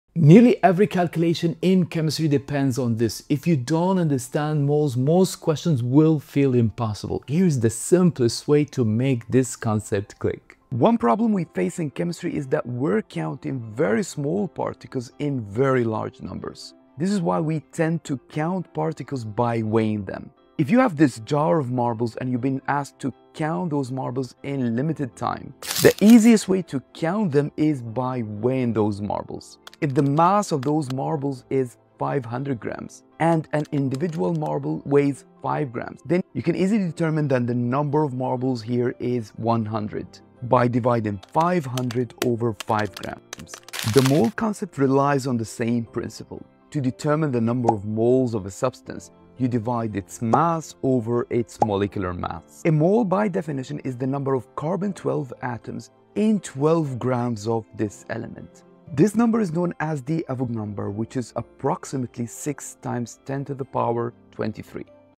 Nearly every calculation in IGCSE & A-level Chemistry depends on this
Get the entire IGCSE chemistry course for 2026 to study at your own pace
the course includes lectures, quizzes, notes and solved past papers

Nearly every calculation in Chemistry depends on this one concept — the mole.
If you don’t understand it, most questions will feel impossible.
In this video, I’ll show you the simplest way to make this topic finally click.

We’ll break it down with clear examples and analogies so you can see how chemists count invisible particles —
and how one simple number, Avogadro’s constant, connects the microscopic world of atoms
to the macroscopic world of reactions, masses, and equations.

Once this concept makes sense, every Chemistry calculation becomes easier —
from reacting masses to empirical formulas and molar concentration.